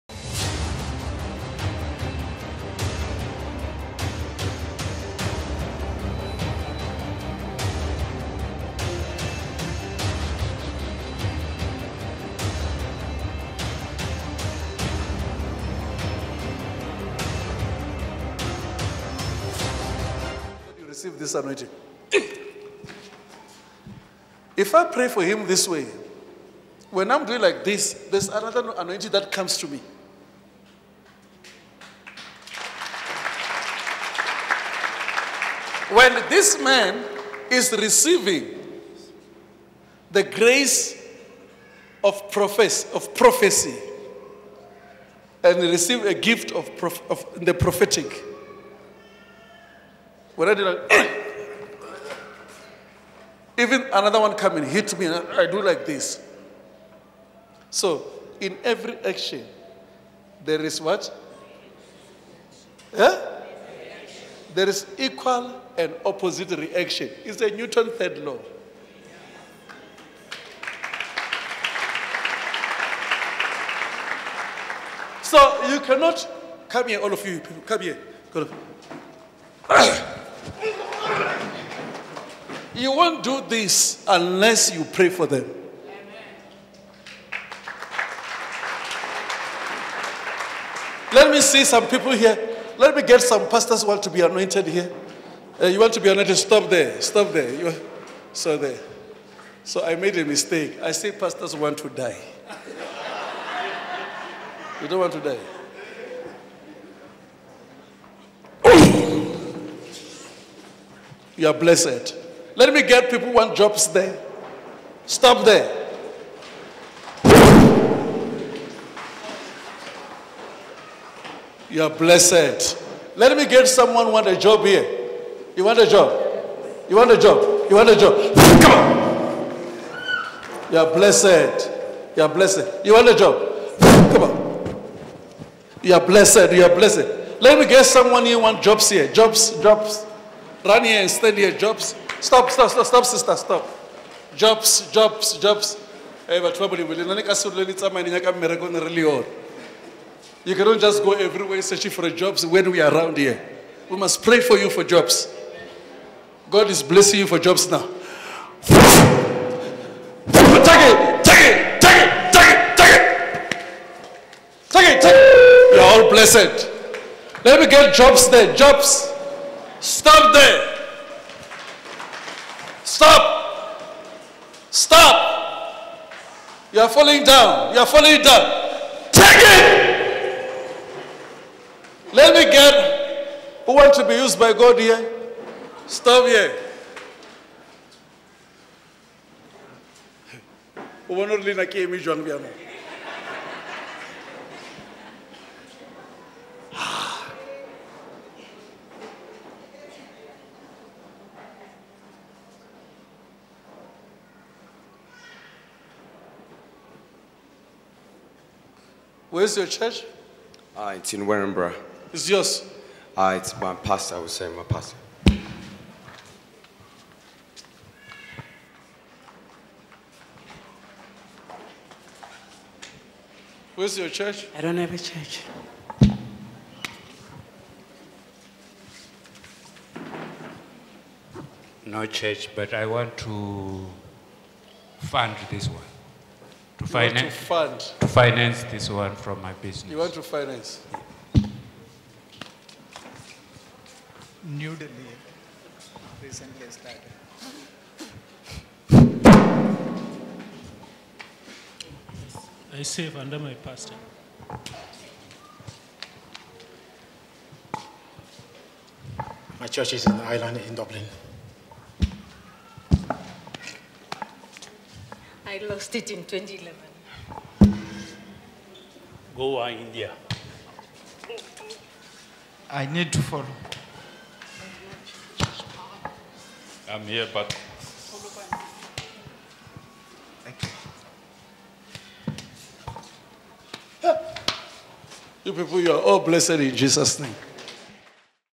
NEWTON’S THIRD LAW AND ANOINTING demonstrated by Apostle JB Makananisa
Apostle JB Makananisa demonstrates how the newton’s third law is related to him when he prays for congregants, showing them how another Anointing comes to him whenever he prays for someone.